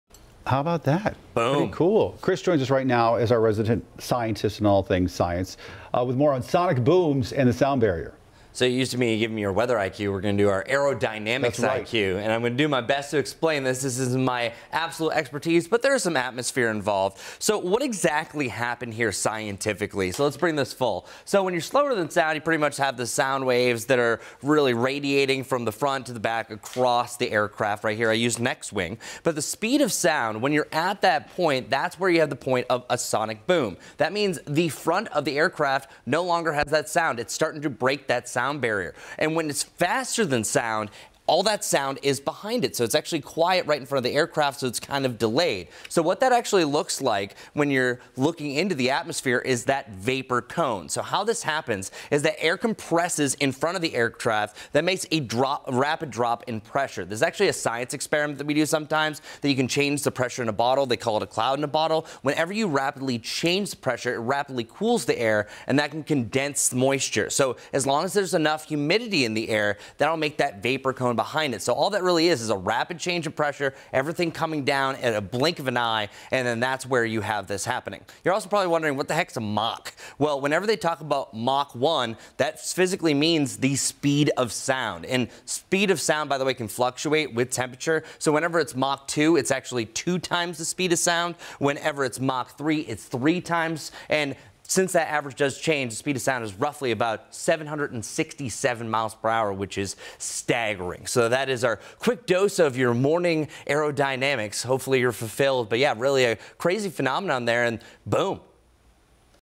Explaining sonic booms and the sound barrier
Meteorologist Chris Mulcahy explains what you need to know about the sound barrier.